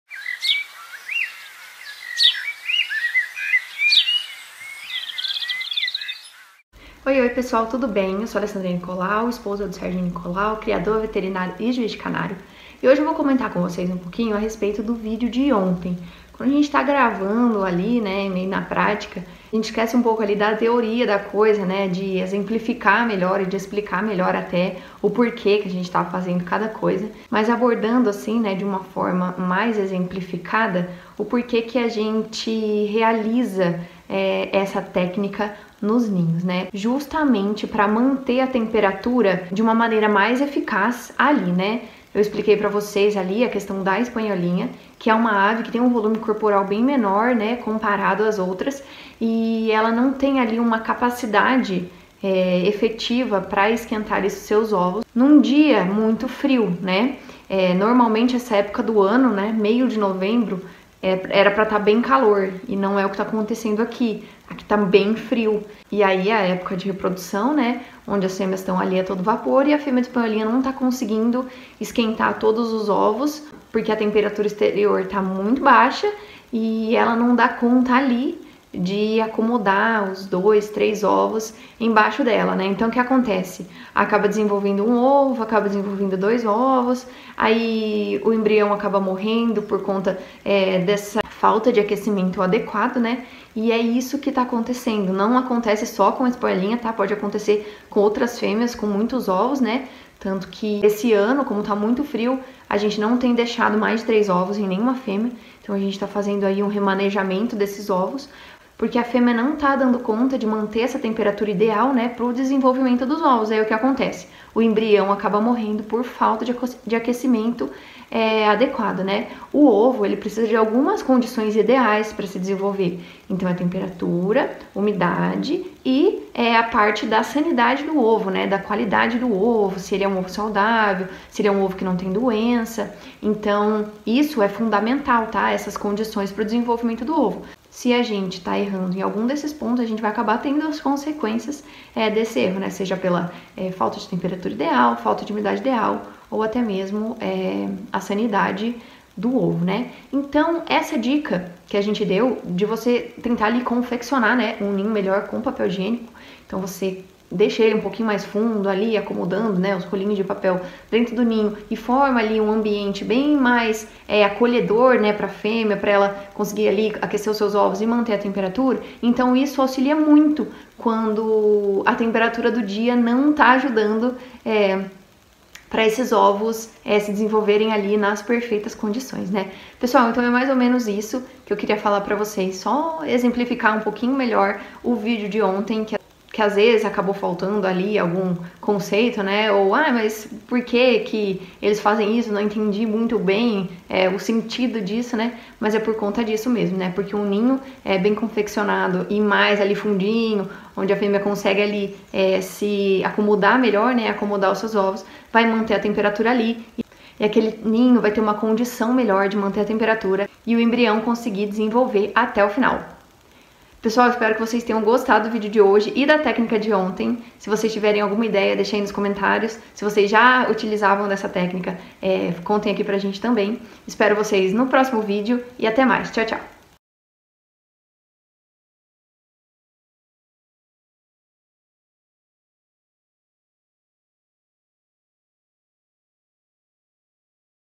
Consequências da falta de condições ideais de choco para os ovos de canário - ovos brancos, gorados
Olá pessoal. O vídeo de ovo é um complemento, uma explicação mais detalhada do vídeo de hoje. Espero que vcs tenham gostado e qualquer dúvida deixem nos comentários que será um prazer conversar com vcs.
Se tiverem alguma sugestão, elas são sempre muito bem-vindas!
Muito obrigada pessoal! E até a próxima!